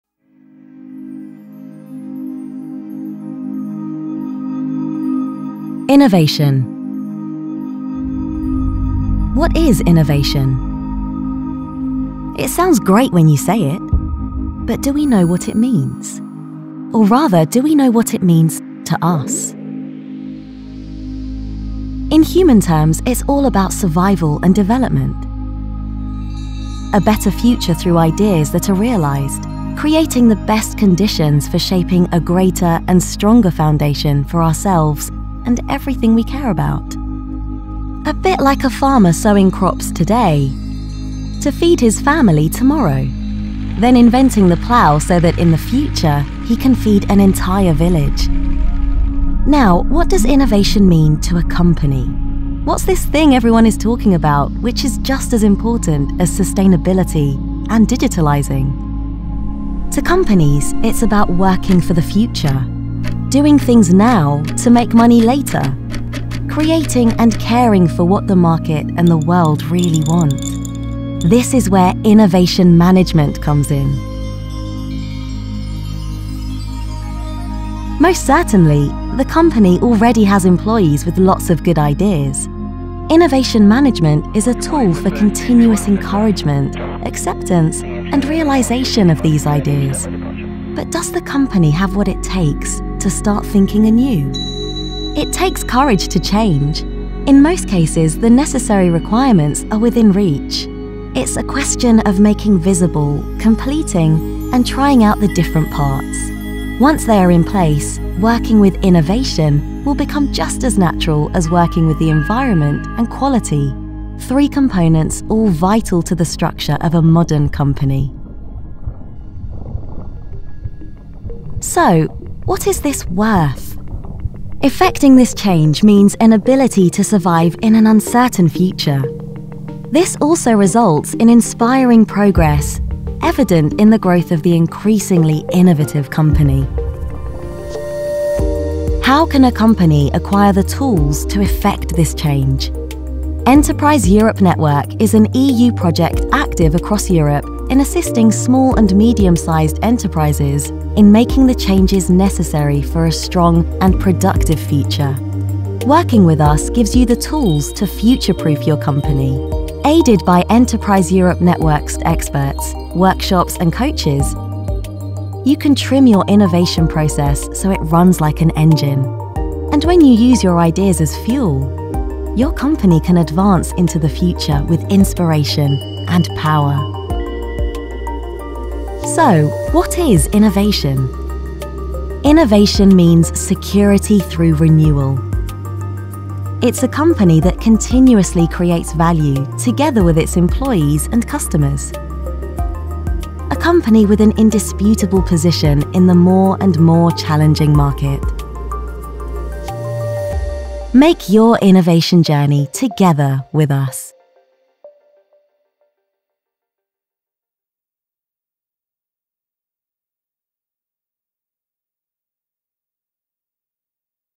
A film about innovation management
What does innovation mean to you? Enterprise Europe Network (EEN) supports small and medium-sized businesses in order to strengthen their capacity to innovate. Essentially it is about finding the paths through innovation management which lets companies act now for future benefits and long-term competitiveness.